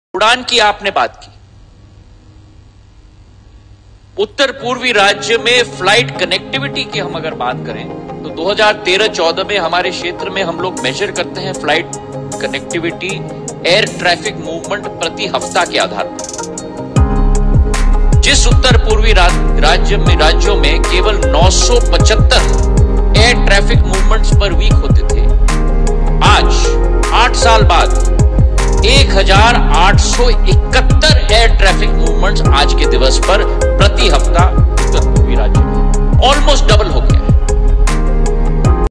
केंद्रीय मंत्री श्रीमंत ज्योतिरादित्य सिंधिया ने उड़ान के संबंधित जानकारियां दी गई#mp
केंद्रीय  मंत्री श्रीमंत ज्योतिरादित्य सिंधिया द्वारा उड़ान संबंधित जानकारियां दी उत्तर-पूर्वी राज्यों में 2013–14 में एयर ट्रैफिक मूवमेंट प्रति सप्ताह जहां 975 थी,देश के यशस्वी *प्रधानमंत्री श्री नरेन्द्र मोदी जी* के 08 वर्ष कार्यकाल पश्चात उड़ान के कारण आज यह बढ़कर 1,871, लगभग दोगुनी हो गई है।
- *श्रीमंत ज्योतिरादित्य सिंधिया जी*, पांचजन्य के 75वें स्थापना दिवस में संबोधन के दौरान।